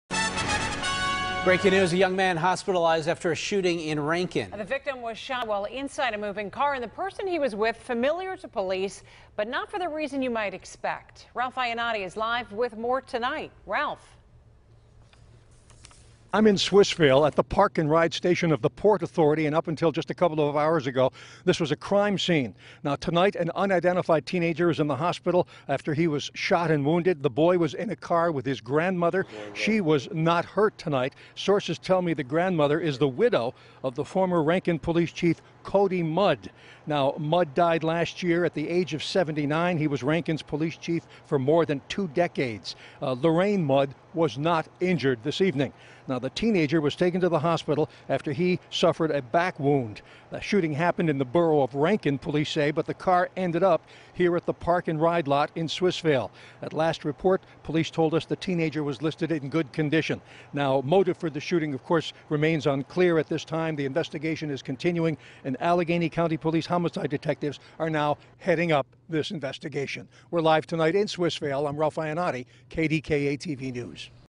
Widow Of Late Police Chief Escapes Injury After Shooting
A young man is in the hospital after he was shot while out with his grandmother in Rankin; KDKA's Ralph Iannotti reports.